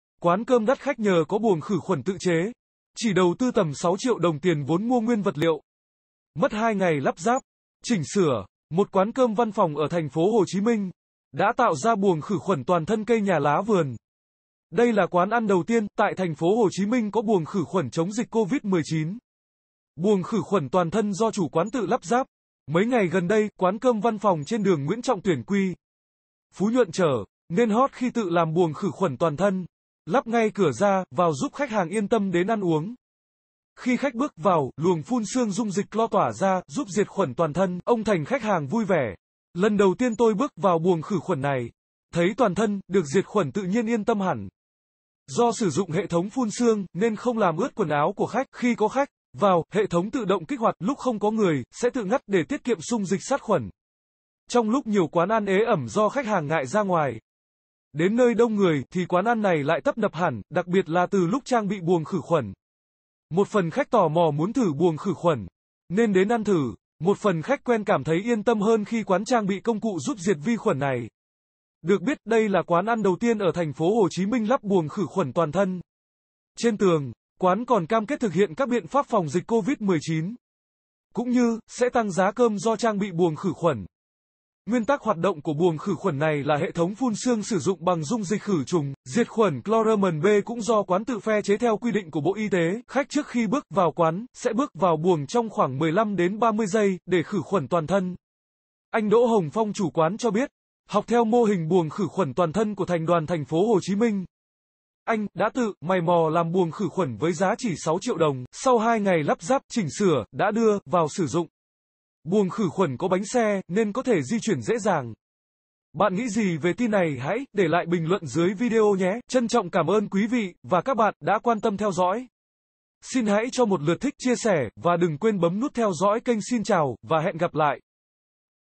Quán cơm đắt khách nhờ có buồng khử khuẩn tự chế
Siêu sale Sen Đỏ dành cho bạn Đăng kí kênh xem tin tức mới nhất Chỉ đầu tư tầm 6 triệu đồng tiền vốn mua nguyên vật liệu, mất 2 ngày lắp ráp, chỉnh sửa, một quán cơm văn phòng ở TPHCM đã tạo ra buồng khử khuẩn toàn thân 'cây nhà lá vườn'. Đây là quán ăn đầu tiên tại TPHCM có buồng khử khuẩn chống dịch Covid-19.Buồng khử khuẩn toàn thân do chủ quán tự lắp ráp
Mấy ngày gần đây, quán cơm văn phòng trên đường Nguyễn Trọng Tuyển (Q.Phú Nhuận) trở nên hot khi tự làm buồng khử khuẩn toàn thân, lắp ngay cửa ra vào giúp khách hàng yên tâm đến ăn uống.Khi khách bước vào, luồng phun sương dung dịch clo tỏa ra, giúp diệt khuẩn toàn thân
Ông Thành (khách hàng) vui vẻ: "Lần đầu tiên tôi bước vào buồng khử khuẩn này
Thấy toàn thân được diệt khuẩn tự nhiên yên tâm hẳn".Do sử dụng hệ thống phun sương nên không làm ướt quần áo của khách
Khi có khách vào, hệ thống tự động kích hoạt, lúc không có người sẽ tự ngắt để tiết kiệm sung dịch sát khuẩn.Trong lúc nhiều quán ăn ế ẩm do khách hàng ngại ra ngoài, đến nơi đông người thì quán ăn này lại tấp nập hẳn, đặc biệt là từ lúc trang bị buồng khử khuẩn.Một phần khách tò mò muốn thử buồng khử khuẩn nên đến ăn thử, một phần khách quen cảm thấy yên tâm hơn khi quán trang bị công cụ giúp diệt vi khuẩn này.Được biết, đây là quán ăn đầu tiên ở TPHCM lắp buồng khử khuẩn toàn thân.Trên tường, quán còn cam kết thực hiện các biện pháp phòng dịch Covid-19, cũng như sẽ tăng giá cơm do trang bị buồng khử khuẩn.Nguyên tắc hoạt động của buồng khử khuẩn này là hệ thống phun sương sử dụng bằng dung dịch khử trùng diệt khuẩn cloramin-B cũng do quán tự phe chế theo quy định của Bộ Y tế
Khách trước khi bước vào quán sẽ bước vào buồng trong khoảng 15-30 giây để khử khuẩn toàn thân.Anh Đỗ Hồng Phong (chủ quán) cho biết, học theo mô hình buồng khử khuẩn toàn thân của Thành đoàn TPHCM, anh đã tự mày mò làm buồng khử khuẩn với giá chỉ 6 triệu đồng
Sau 2 ngày lắp ráp, chỉnh sửa đã đưa vào sử dụng
Buồng khử khuẩn có bánh xe nên có thể di chuyển dễ dàng.